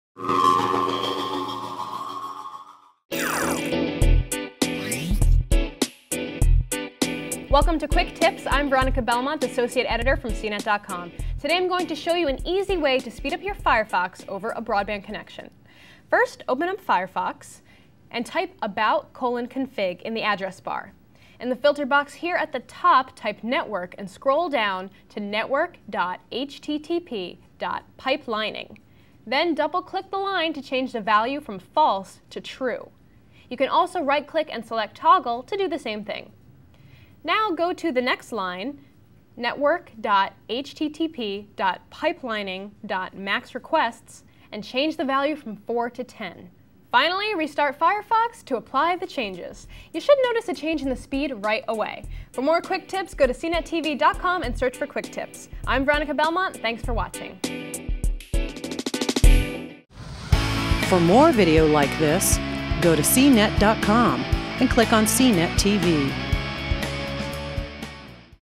Speedup Firefox
Making couple of changes in Firefox Internet Browser's configuration, you can make it work faster.